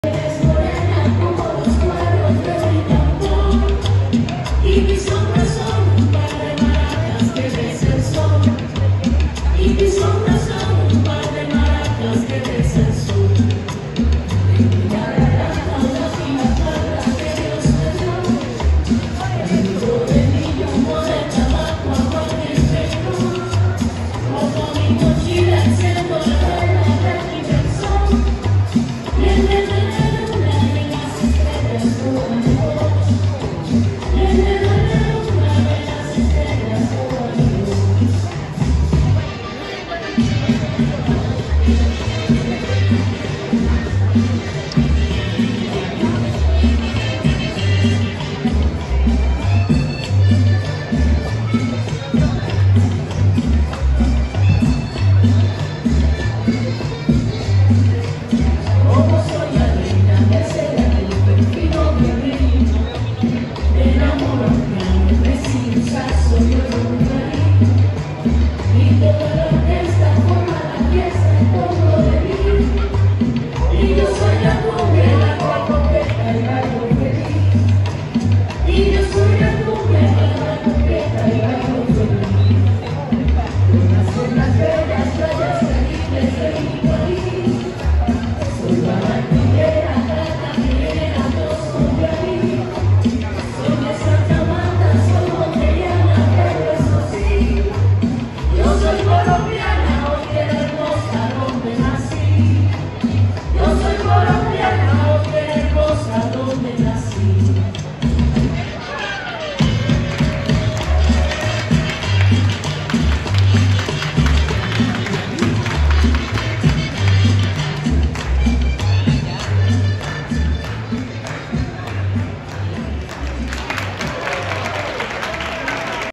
AEROPUERTO INTERNACIONAL JOSE MARIA CORDOBA, RIONEGRO. ANTIOQUIA.
Los trabajadores de la limpieza quisieron hacer esta coreografia para impusar la campaña de mantener el aeropuerto limpio sin basura.